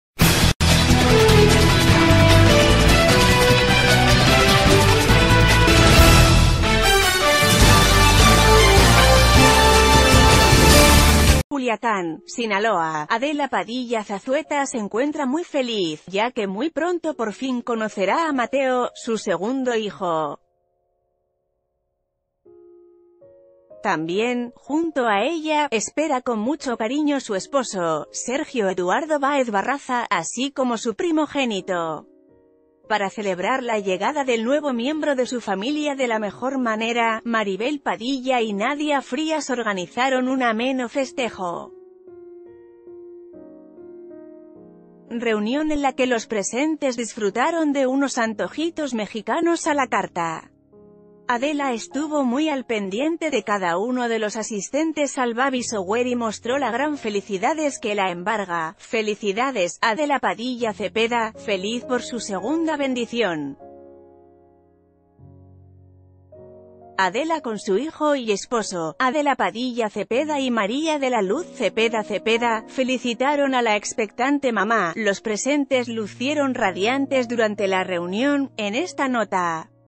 Espera a su segundo hijo, Mateo
Culiacán, Sinaloa.- Adela Padilla Zazueta se encuentra muy feliz, ya que muy pronto por fin conocerá a Mateo, su segundo hijo. También, junto a ella, espera con mucho cariño su esposo, Sergio Eduardo Báez Barraza, así como su primogénito. Para celebrar la llegada del nuevo miembro de su familia de l...